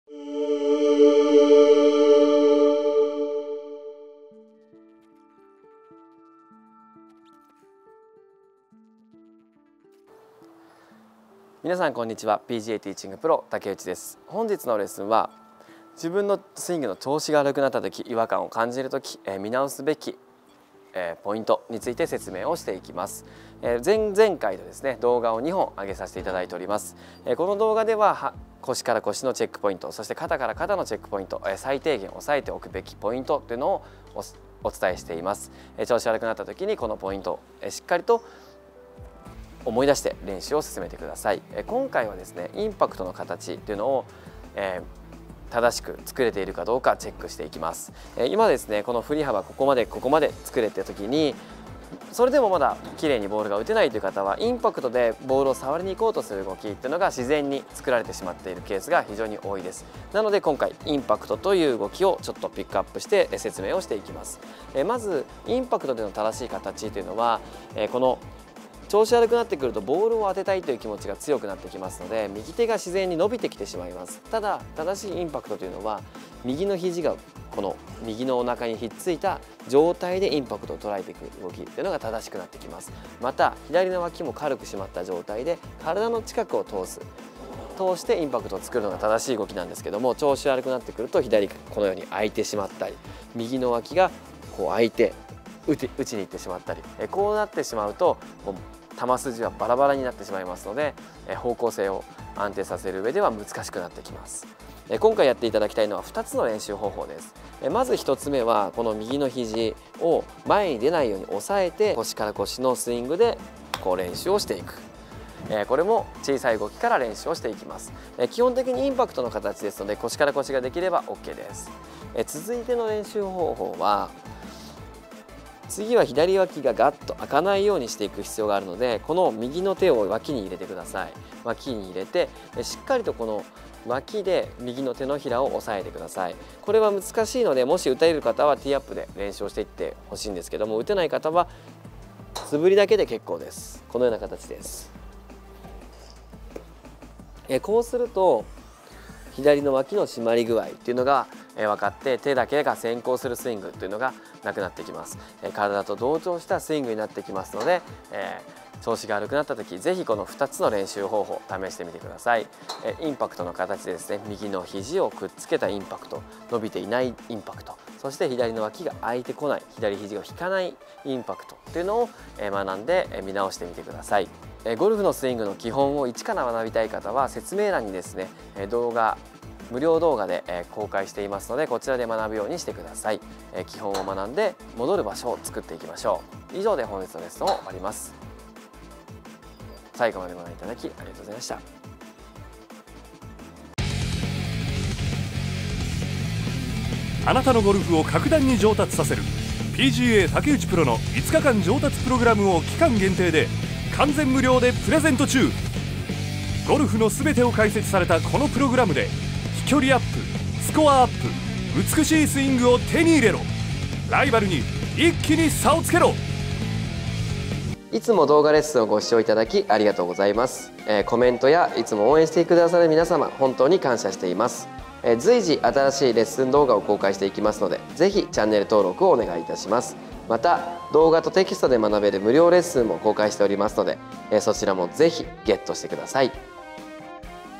スイングの不調や違和感はまず基本に戻るべし！ 基本見直し編３
【期間限定】完全無料で動画とテキストで学べるレッスンプログラムをプレゼント中！

スイングの不調や違和感を覚えた際は、基本をチェック！

◆レッスン動画のプレイリスト

◆竹内雄一郎プロのレッスンDVDが大好評発売中です！